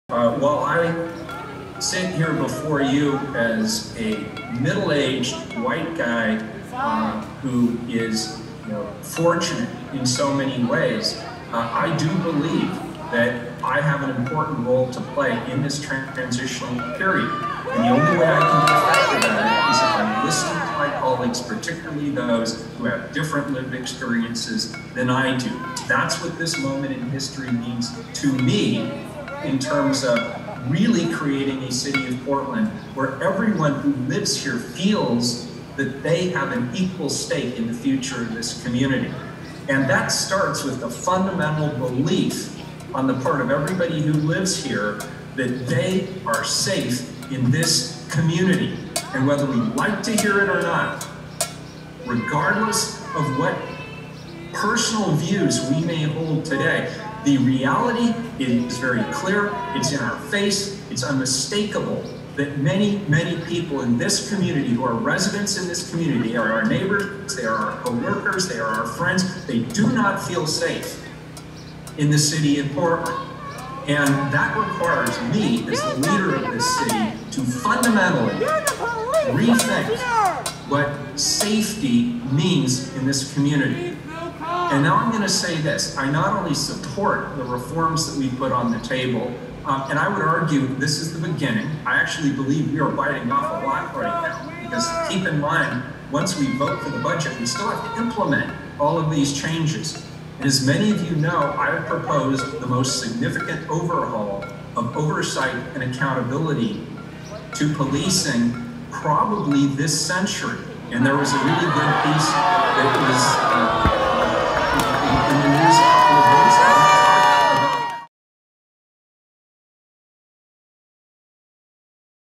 Mayor Ted Wheeler addresses protesters gathered outside his apartment
An estimated 150 to 200 protesters assembled while Mayor Ted Wheeler participated in a virtual city council meeting. The demonstrators wanted their voices heard as Wheeler and his colleagues are voting on the city budget.

Wheeler acknowledged the protesters during the meeting.

“I hear you,” he said. “And I see you. And I am with you on this challenging path ahead.”